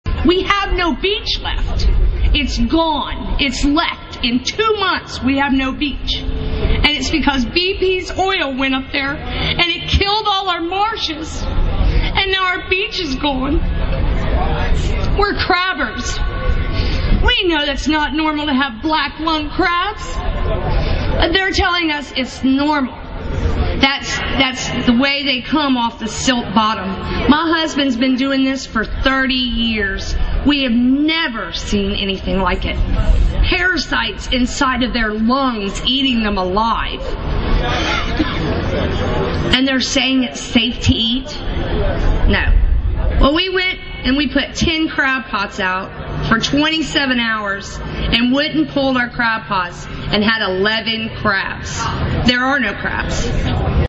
"There are no crabs" -- "Parasites inside of their lungs eating them alive"
Lorrie Williams, Rally for the Truth, November 20, 2010.